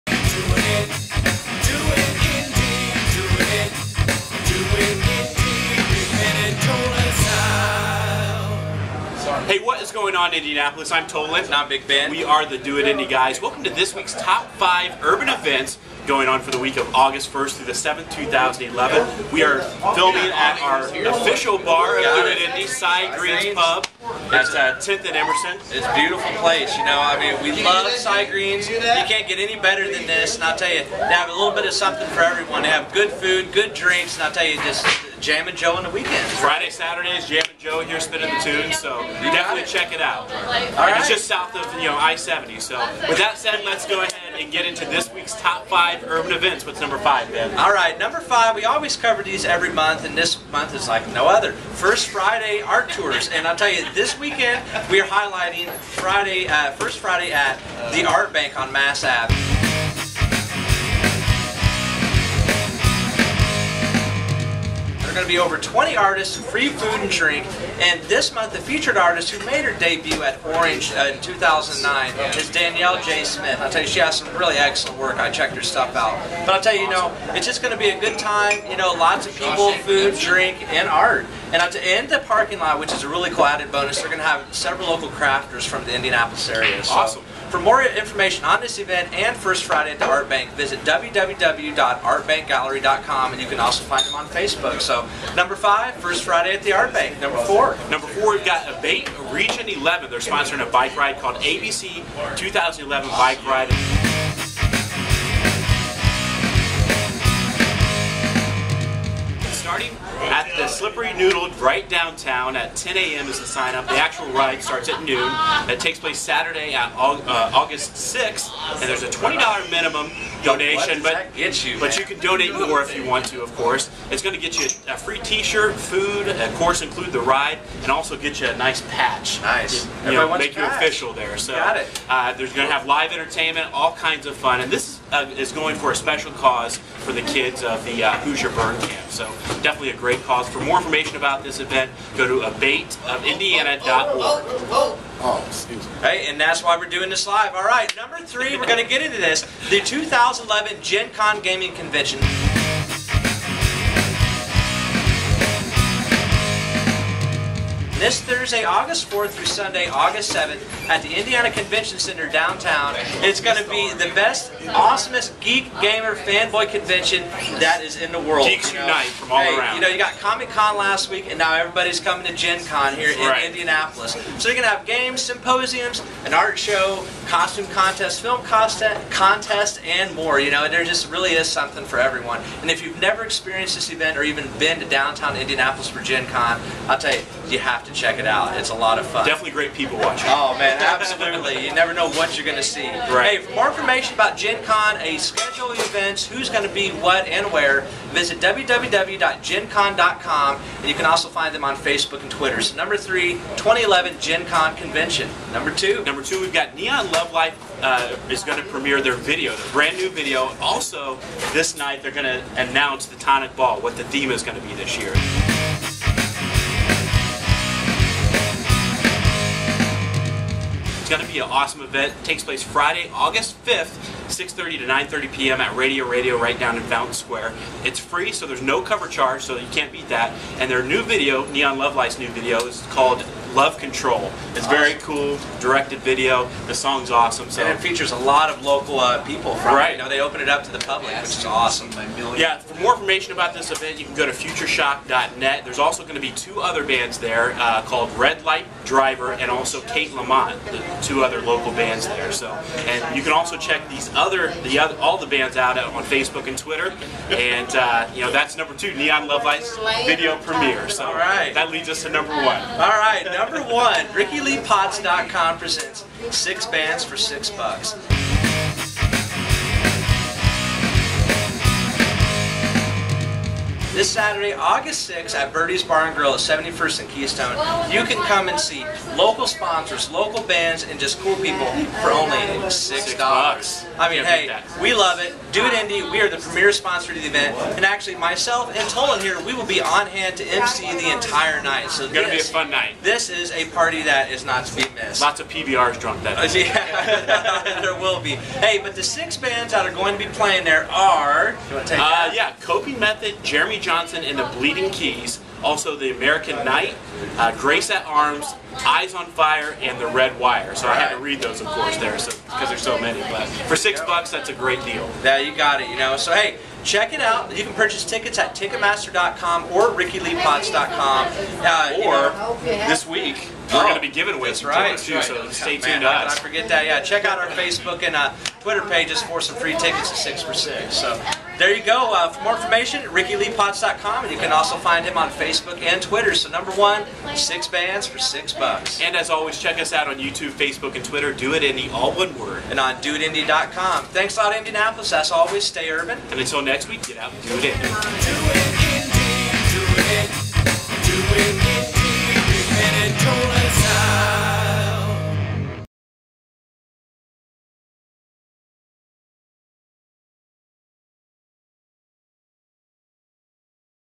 DoItIndy, Vol. 38, August 1-7, 2011
Welcome to August Indianapolis! The weather is smoking hot in the 90's and so are this week's events. Live music, art, motorcycle charity ride, GEN CON, Neon Love Life and did I mention live music at six bands 4 six bucks?!